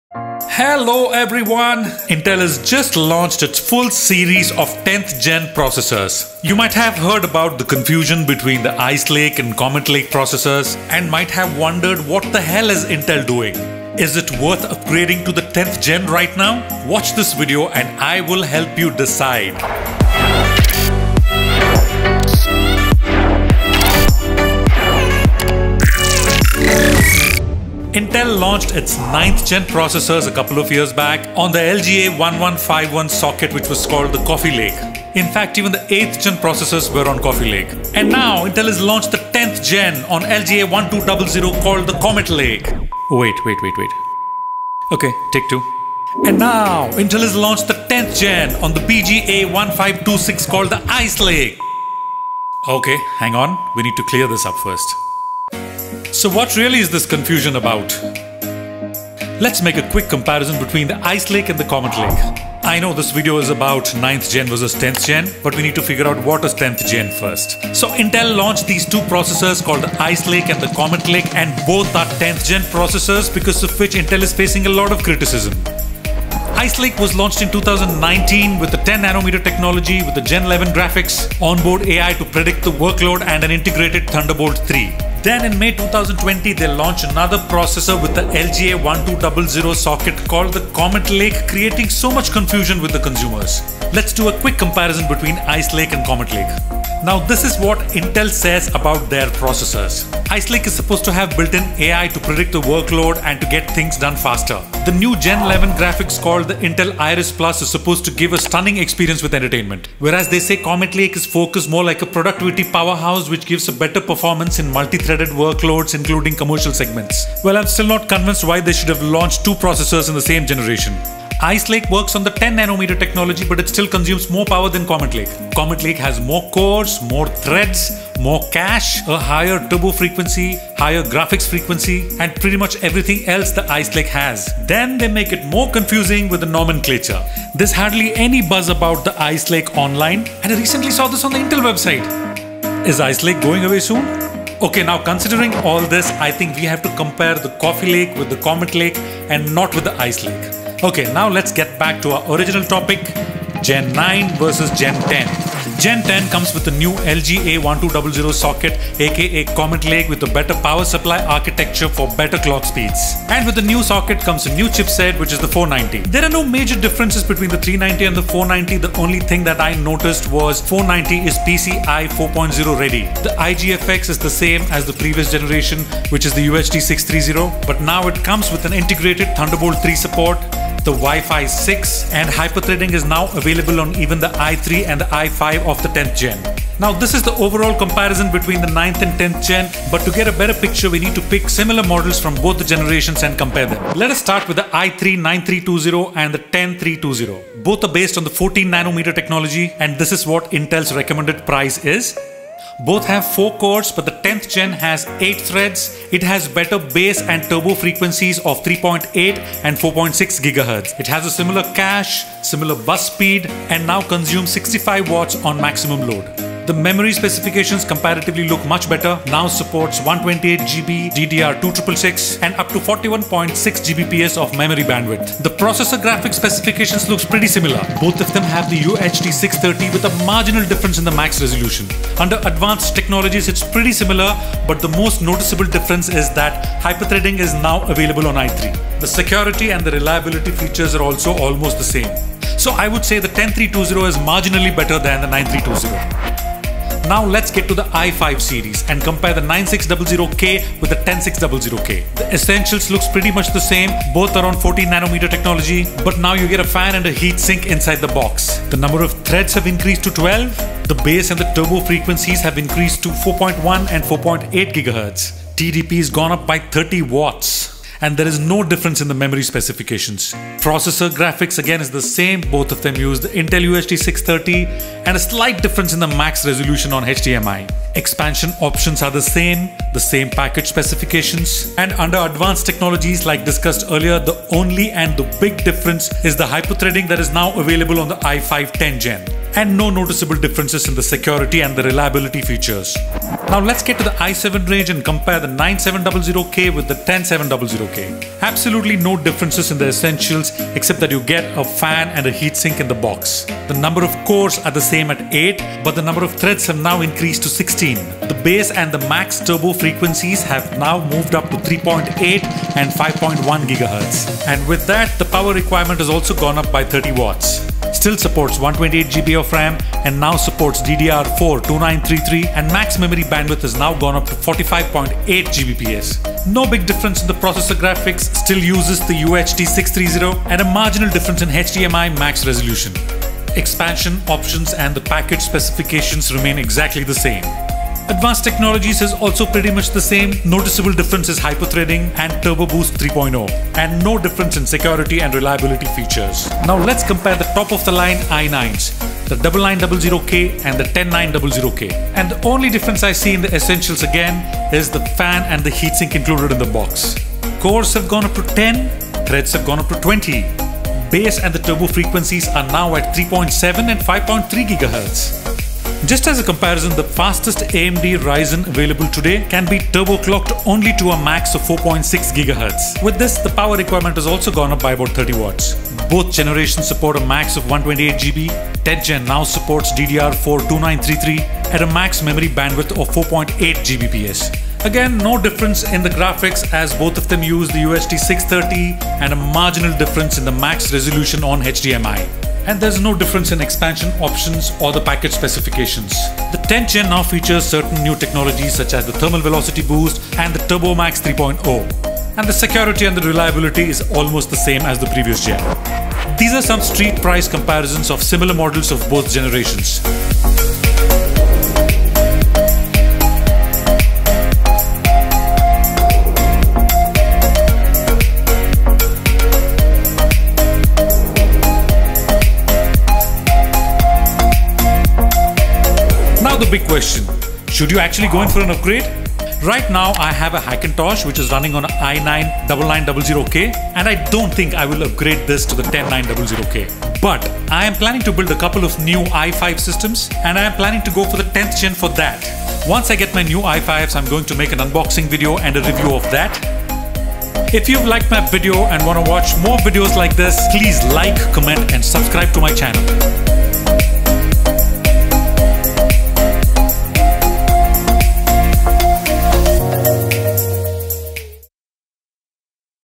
INTEL CORE 9th gen vs 10th gen. Has intel lost it?? Coffeelake vs Cometlake vs Icelake
ntel hast just launched its full series of 10th gen processors. You might have heard about the confusion between the ice lake and comet lake processors and might have wondered what the hell is Intel doing.Is it worth upgrading to the 10th gen.

Intel launched its 9th gen processor a couple of years back on the lga1151 socket with was called the coffee lake. In fact even the 8th gen processor was on coffee lake. Let's make a quick comparison between the ice lake and the comet Lake. I know this video is about 9th gen vs 10th gen but  we need to figure out what is 10th gen first. There are 2 processors called ice lake and the comet lake and both the 10th gen processors because of this Intel is facing a lot of criticism. Ice lake was launched in 2019 with 10 NM technology with the 11 graphics on board a has AI to predict workload and an integrated thunderbolt 3. In 2020 they launched another processor with LGA 1200 socket called the comet Lake creating so much confusion with the consumer. Let’s do a quick comparison between ice lake and comet Lake. Intel tells us about their processes have built-in AI to predict to workload and to get things done faster the new gen11 graphics called the Intel iris Plus is supposed to give a stunning experience with entertainment. Where as the comet lake is focussed more like a production power house which gives better performance in multithreaded workloads including commercial segments. Ice lake is on 10 NM technology but it consumes more power than Comet lake. Then they make it more confusing with the nomenclature. There was hardly any Buzz about the ice Lake. now considering all this I think we have to compare with the comet lake and not with ice Lake. Let's get back to original topic Get 9 vs 10 gen. Get 10 comes with the new LGA 1200 socket with the better power supply architecture for better clock speeds. New chipset which is the Z490. It has the same IGFX as previous generation which is the UHD 630 but now it comes with an integrated thunderbolt 3 support, the Wi-Fi 6 and hyperthreading is now available on even the i3 and i5 of the 10th gen.